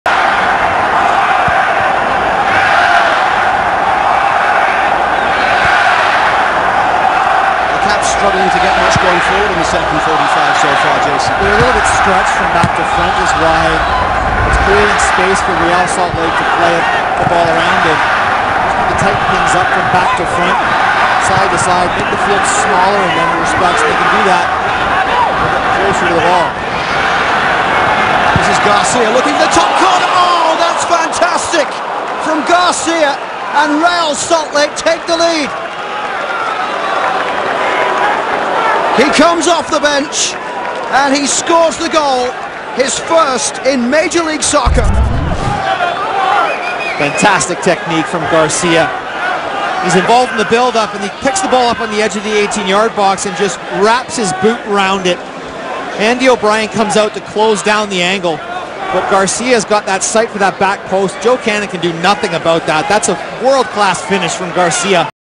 GOAL: Garcia launches from distance | Vancouver Whitecaps vs. Real Salt Lake
Goal! Vancouver Whitecaps 0, Real Salt Lake 1. Olmes Garcia (Real Salt Lake) right footed shot from outside the box to the top right corner.